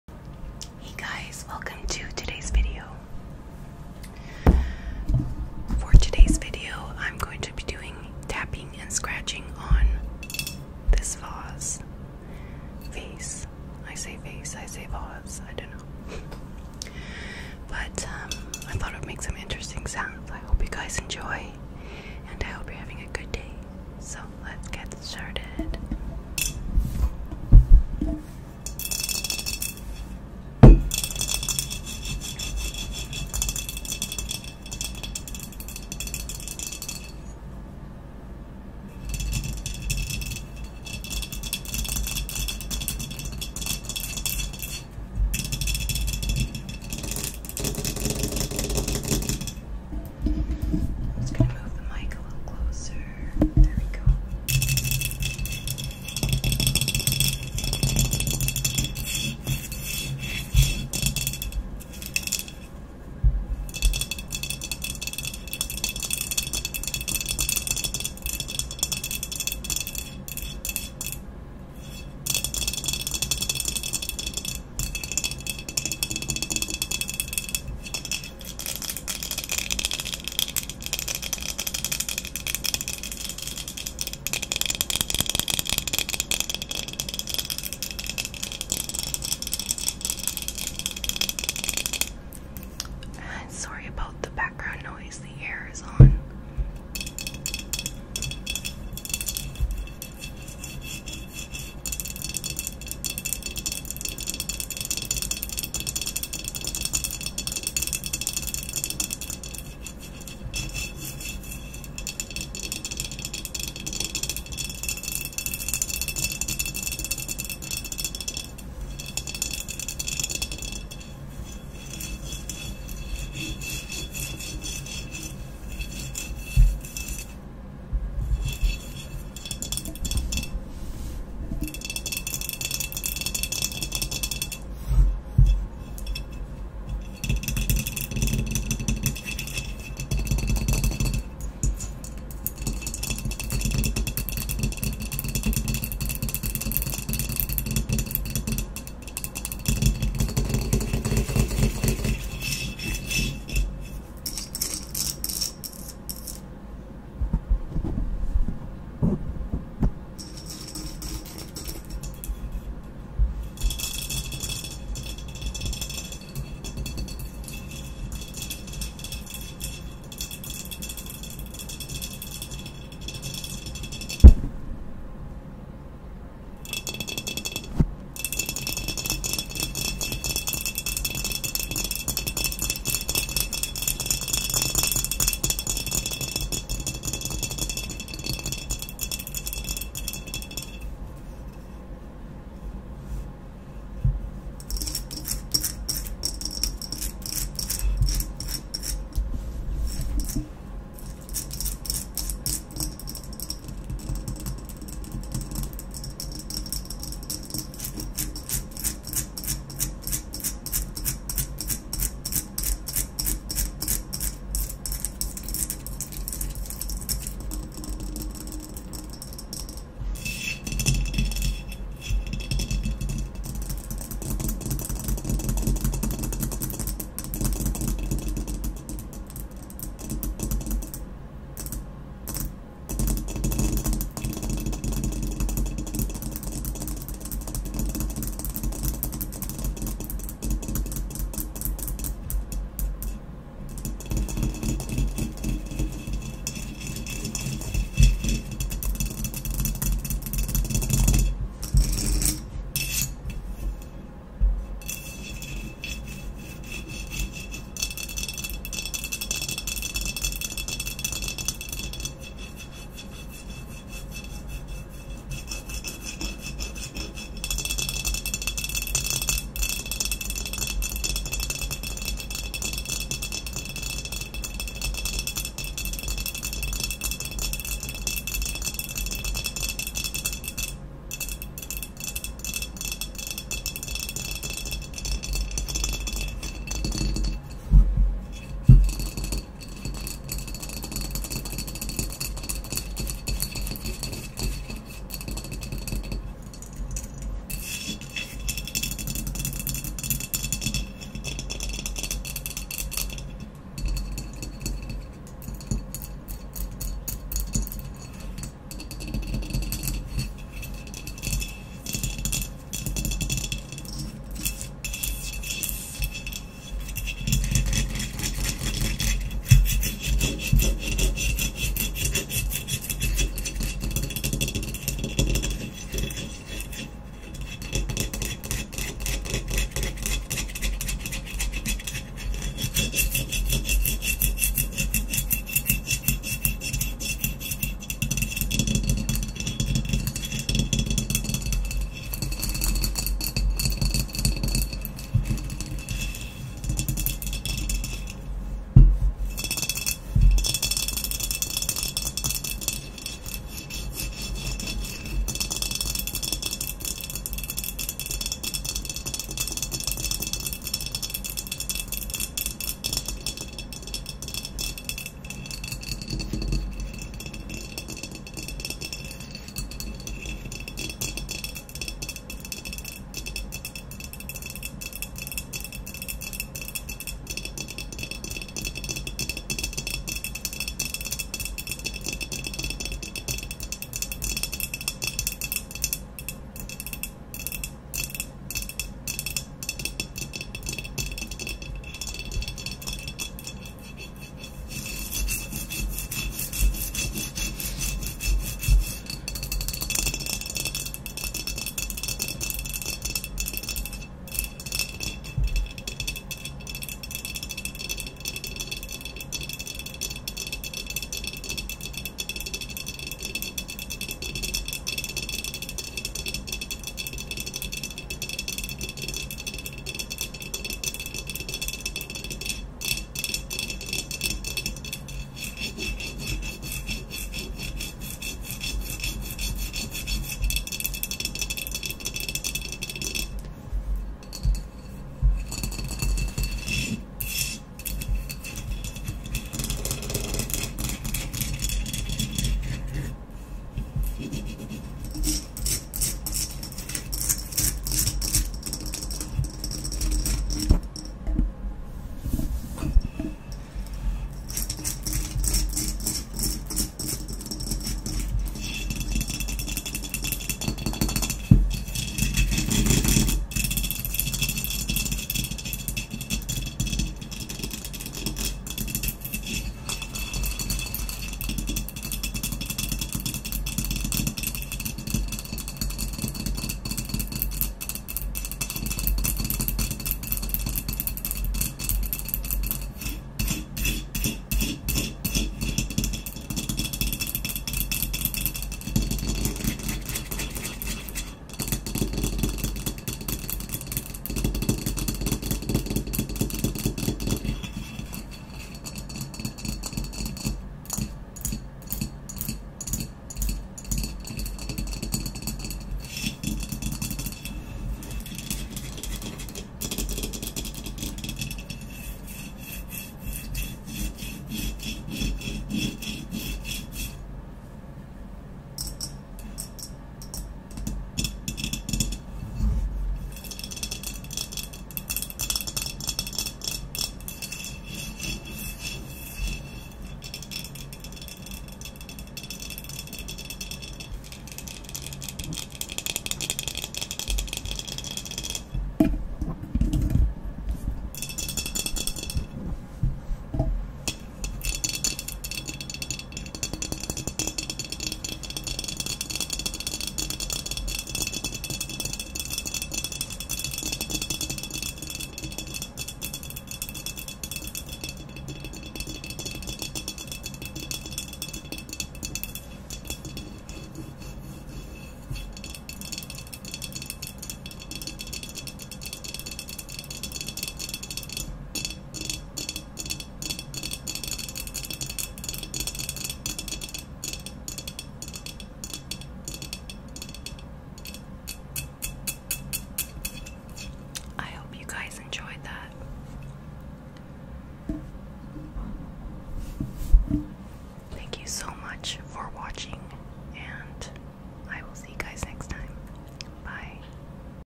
ASMR Fast Tap And Scratch On A Vase
Hi guys, I hope you enjoy today’s video. I thought this vase would make some nice tapping and scratching sounds.